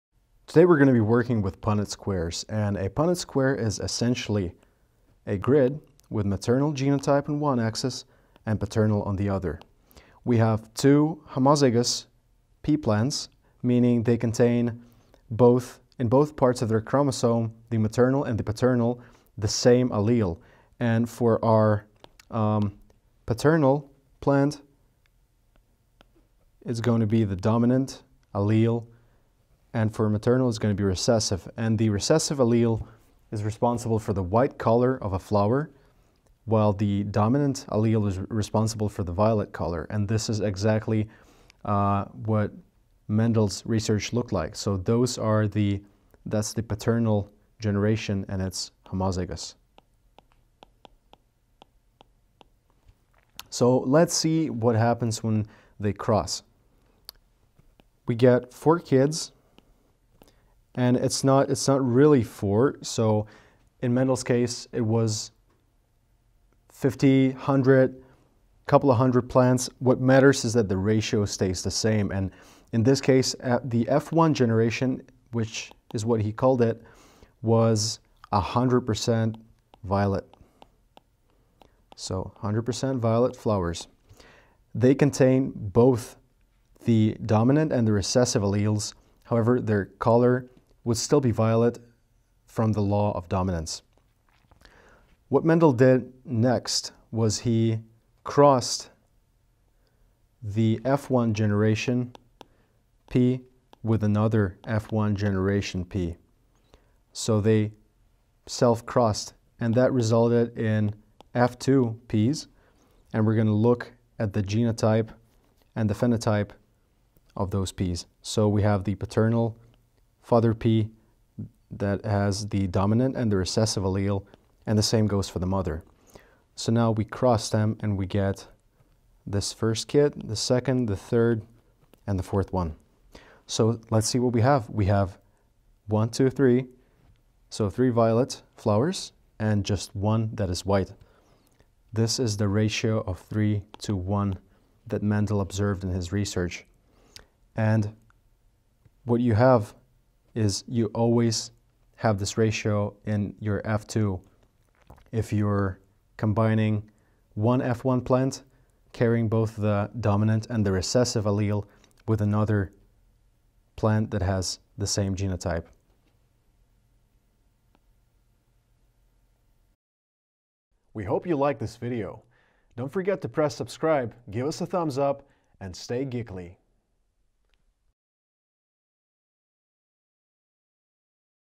GeeklyHub Punnett Squares: How to Predict Offspring Features
😎 Struggling with your college tasks? Get your homework under control. Check out GeeklyHub and get yourself a Geek in a few clicks.

Hi there and welcome to GeeklyEDU by GeeklyHub Biology! In this video we will look at basic Punnett Squares. They are used to check the probability of an offspring to have its parents' traits. We will check the influence of dominant and recessive alleles in breeding.